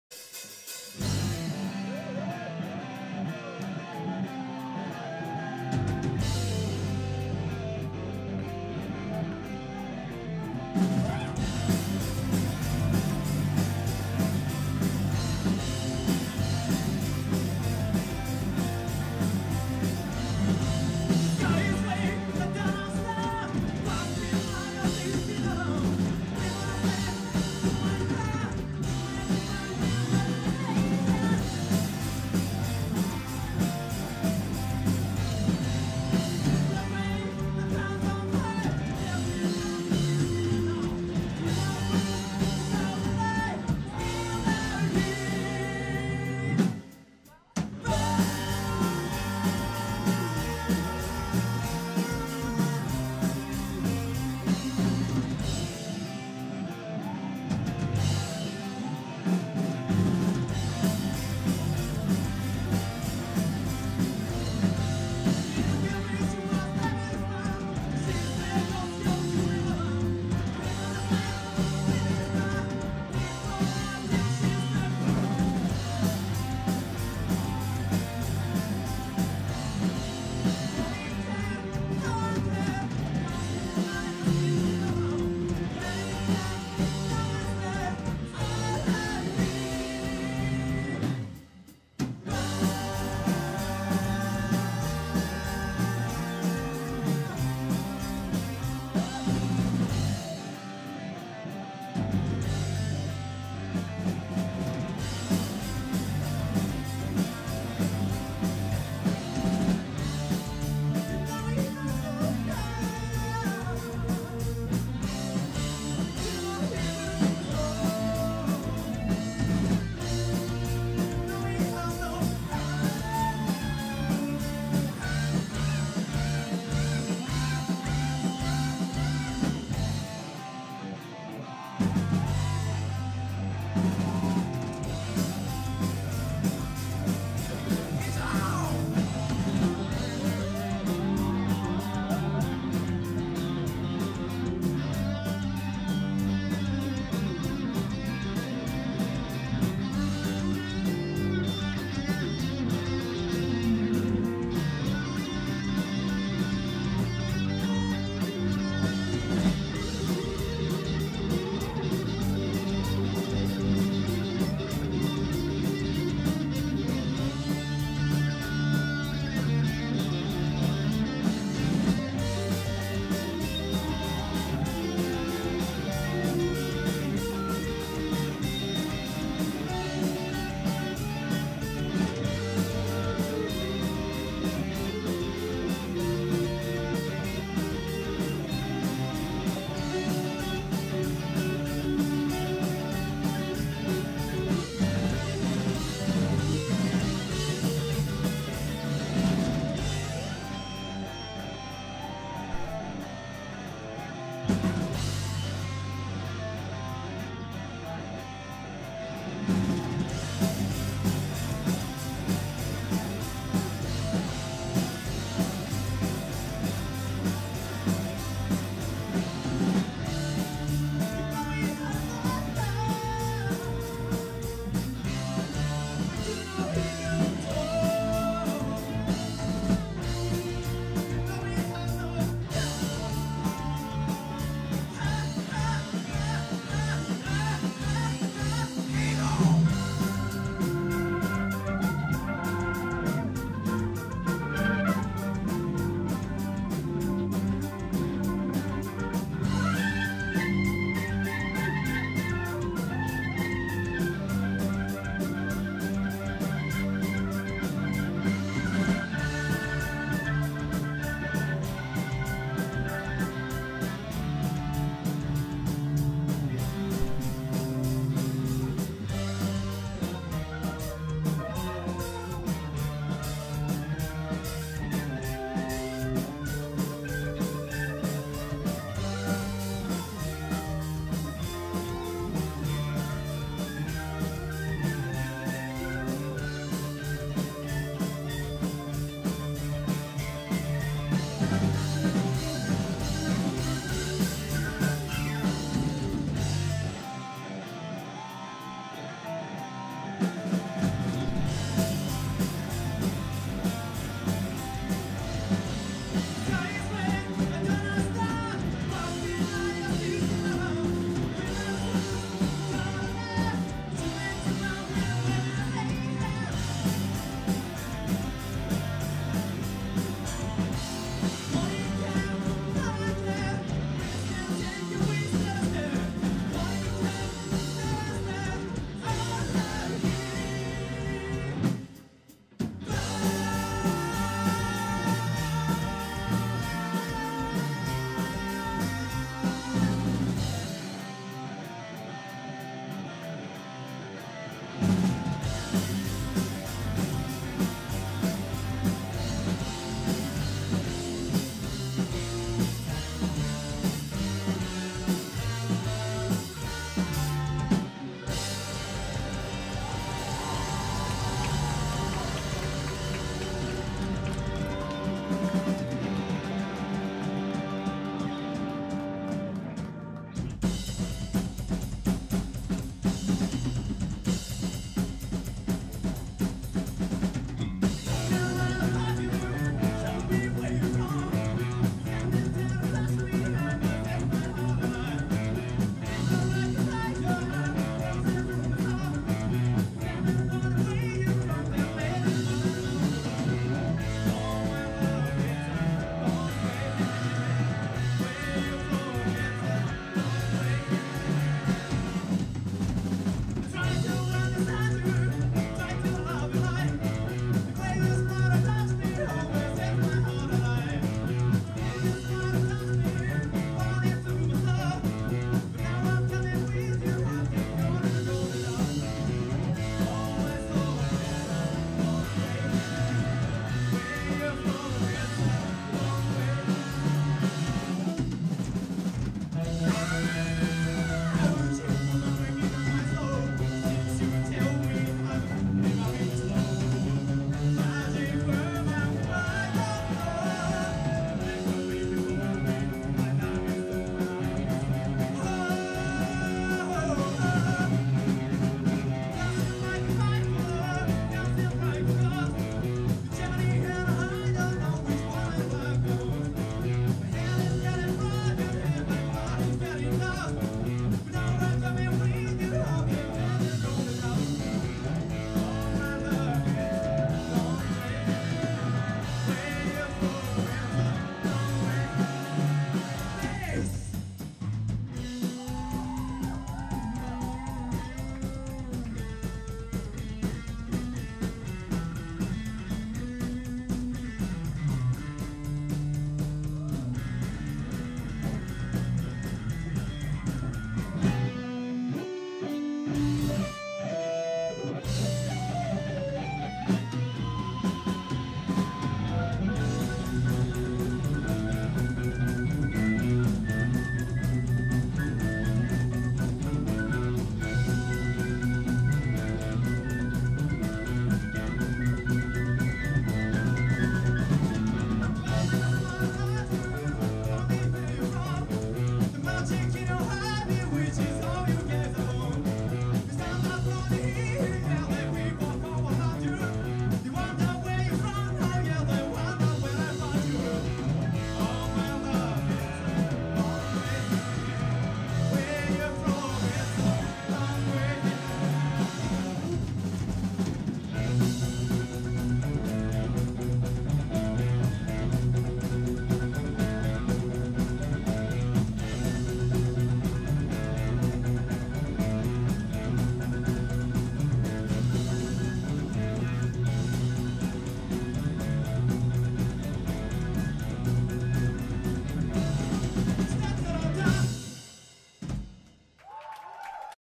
Deep Purple#1@12.12BLITZ対バン
一橋大学BLITZさんとの恒例の対バン＠吉祥寺シルバーエレファント